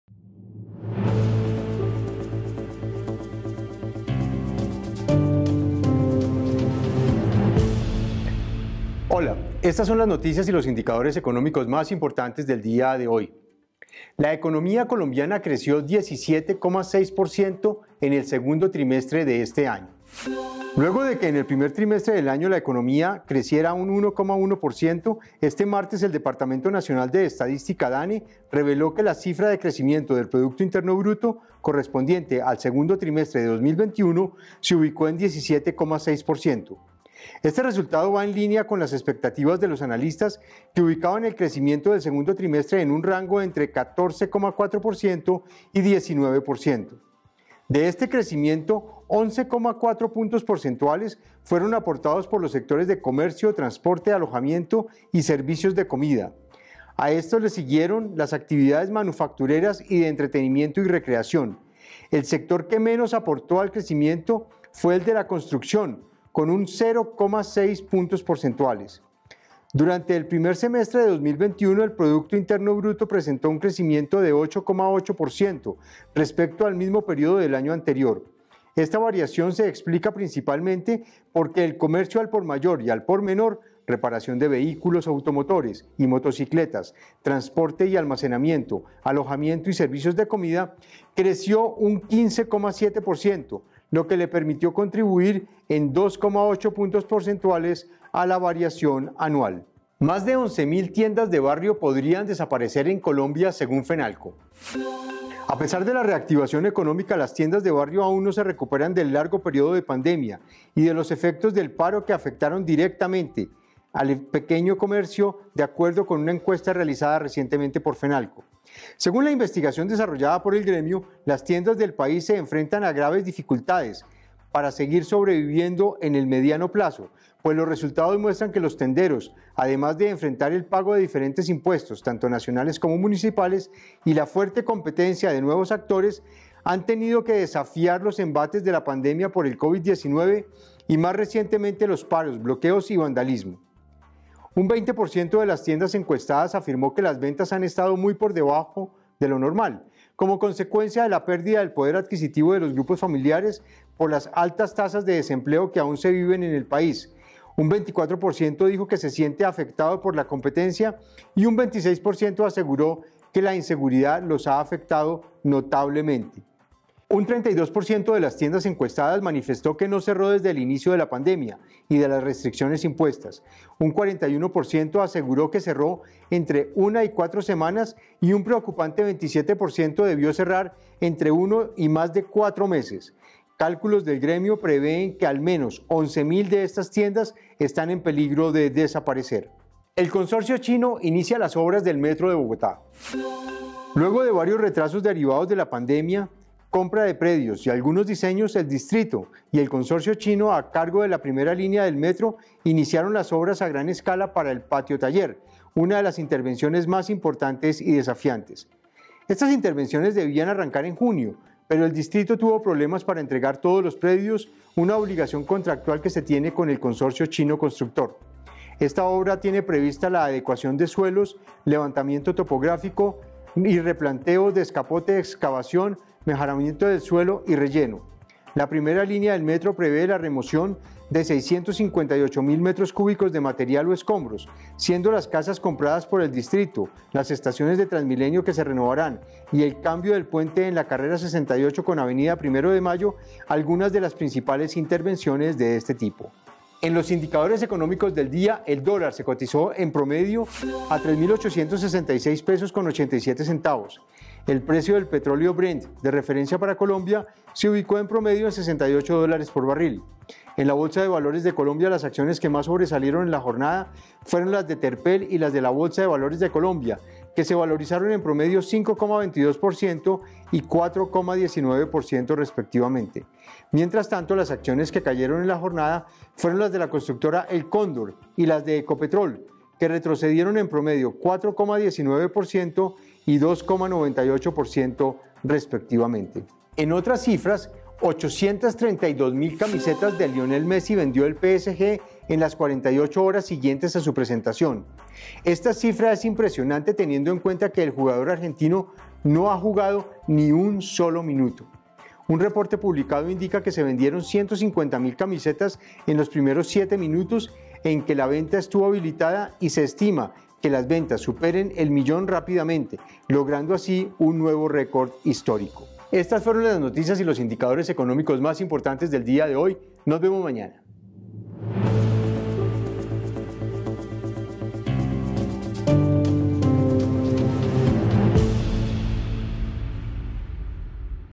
Economía colombiana creció 17,6% en el segundo trimestre de 2021
El PIB de Colombia para el segundo trimestre se ubicó en 17,6%, en línea con las expectativas de los analistas. Además iniciaron las obras de la primera línea del metro de Bogotá. Estos son los